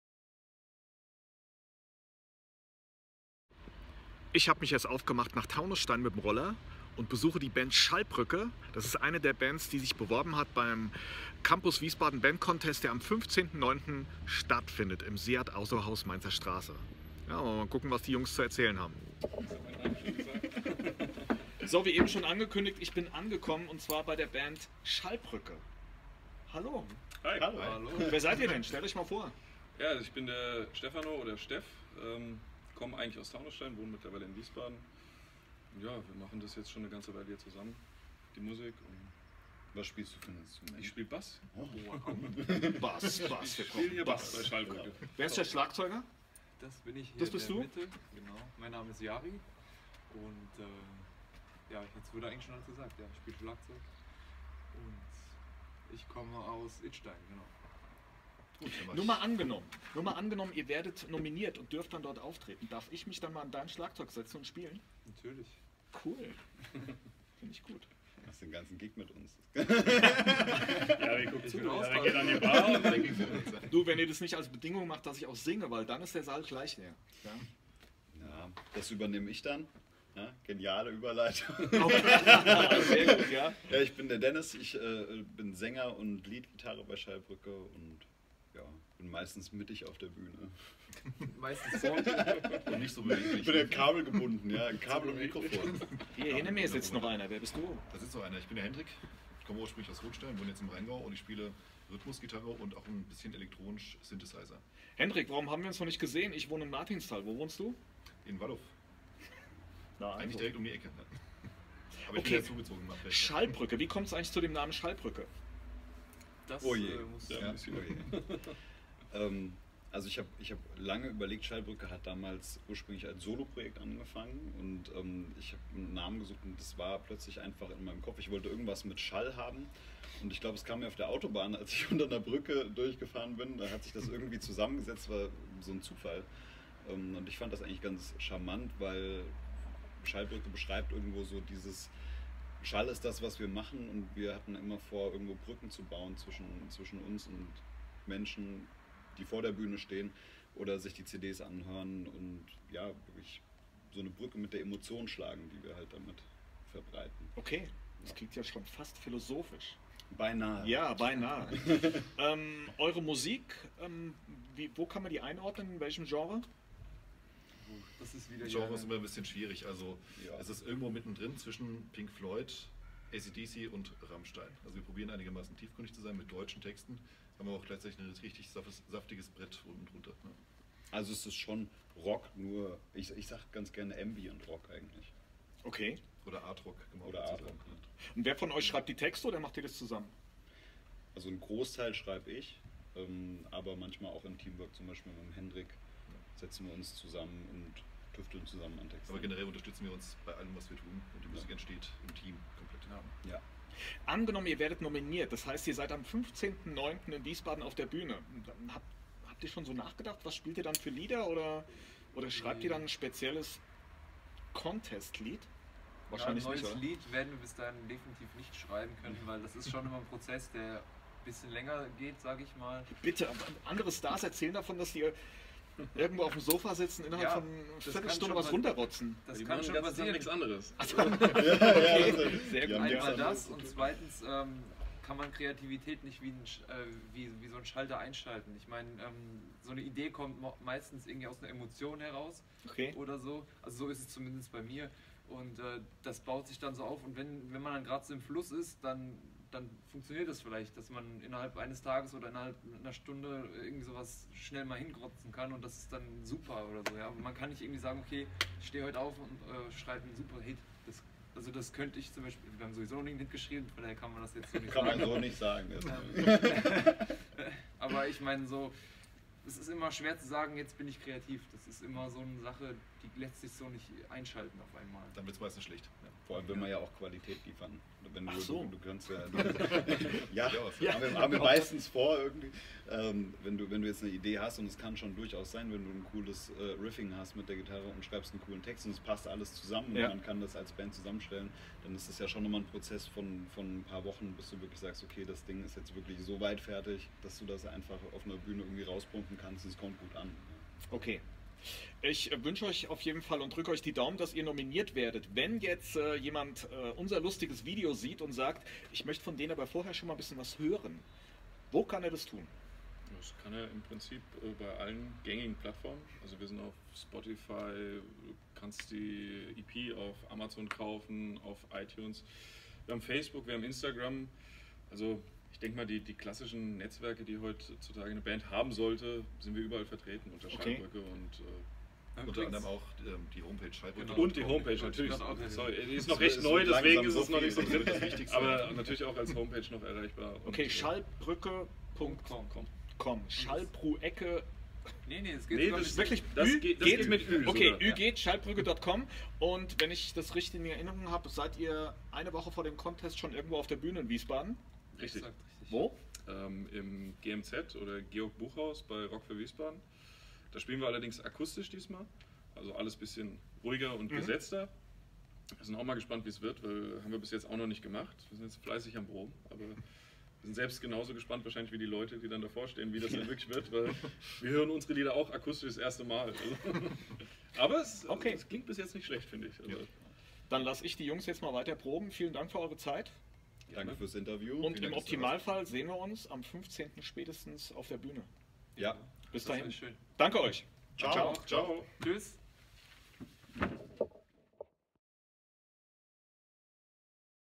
Die Band Schallbrücke
Eine derer Bands, die sich beim Campus Wiesbaden Bandcontest beworben haben. Und nun natürlich die Frage. werden sie nominiert oder. ?